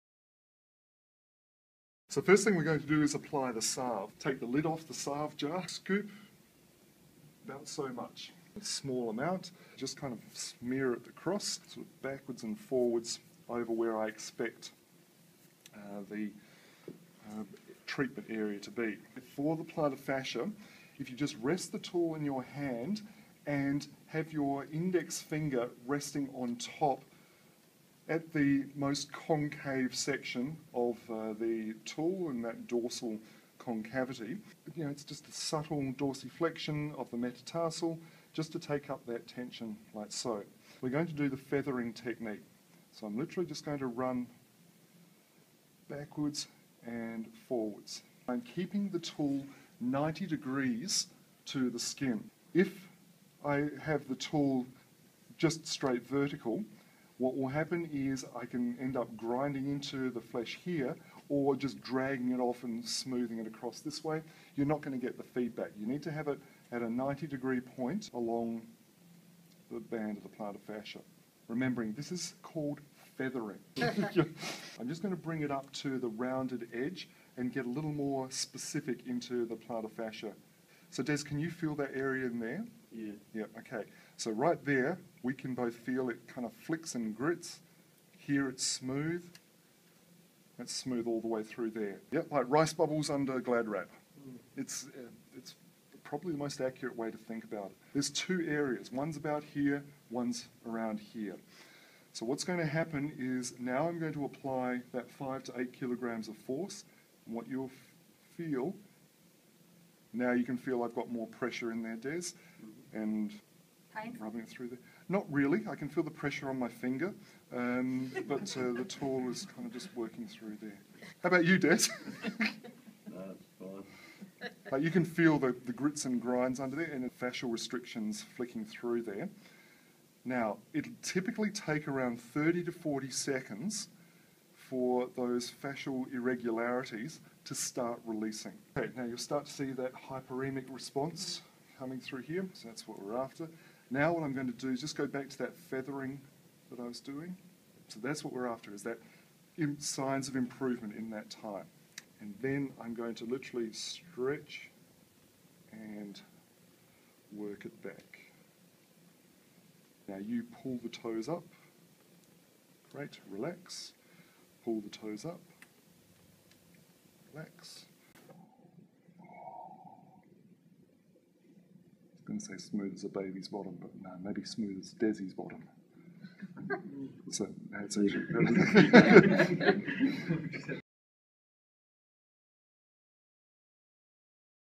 TAR Pl Fas
Tool Assisted Release (TAR) techniques for releasing fascial restrictions of the plantar fascia.